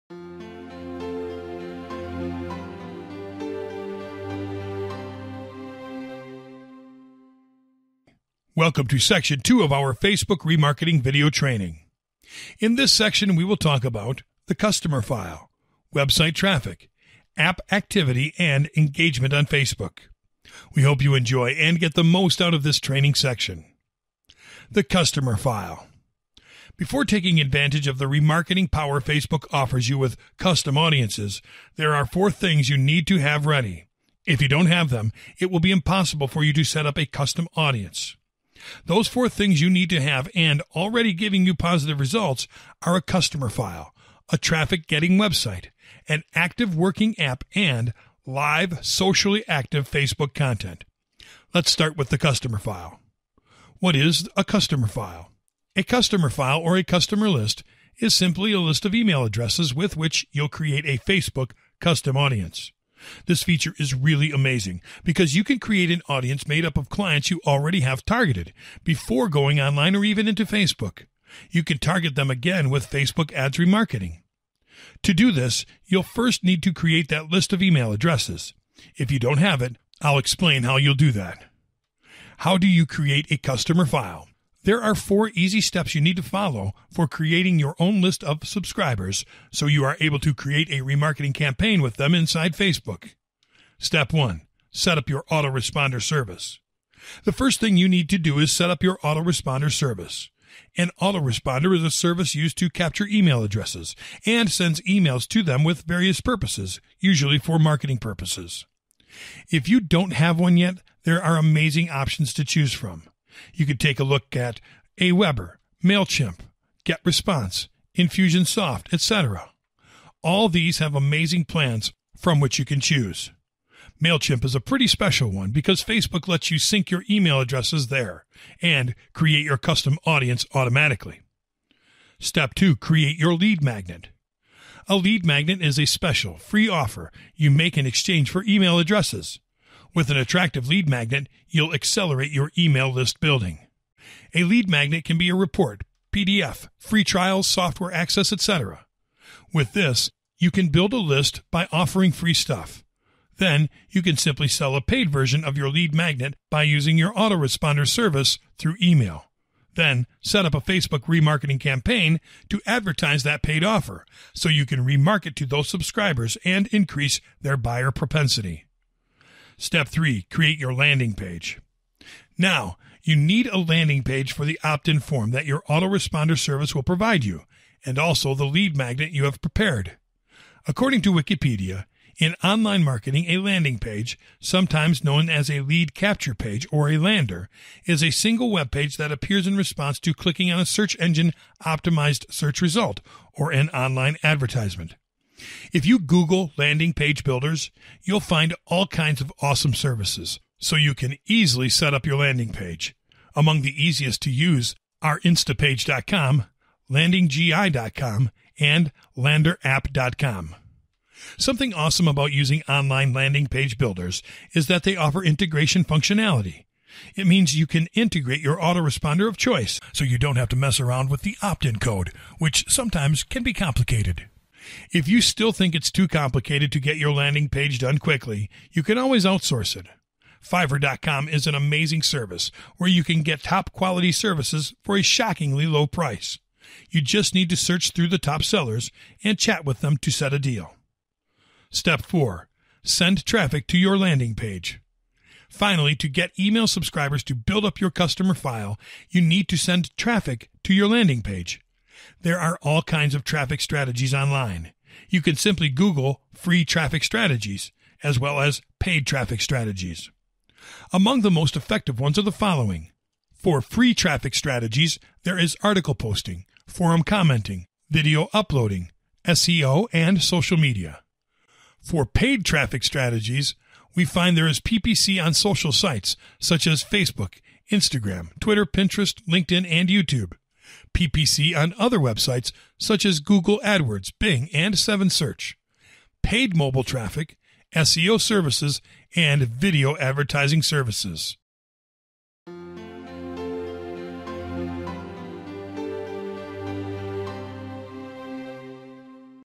Facebook Remarketing Made Easy 2.0 - Section 2 - Setting Up Everything Before Choosing Audience
How to facebook remarketing!
Learn more about Facebook ads retargeting and get your marketing checklist